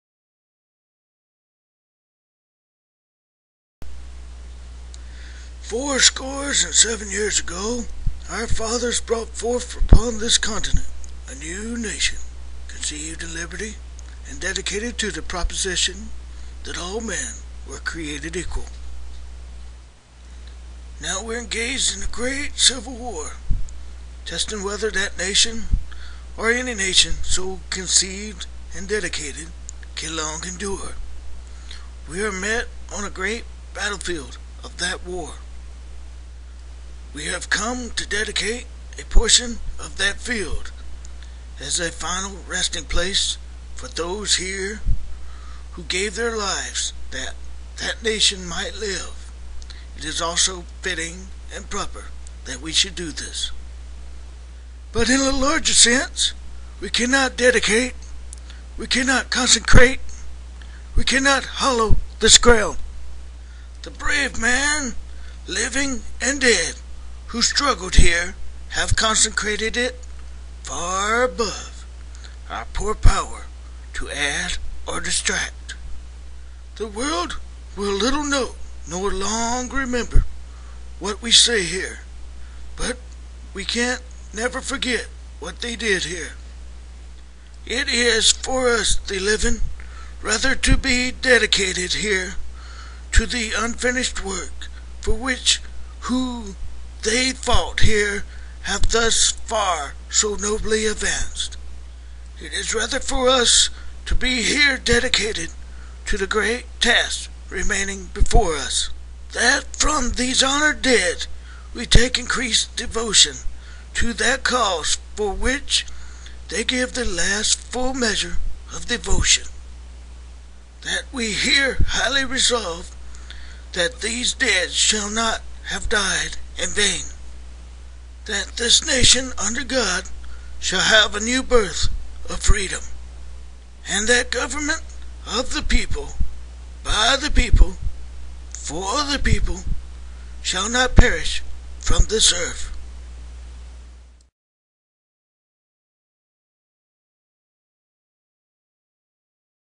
The lost Gettysburg address ?
A pictorial of the gettysburg address with voice and moveing picture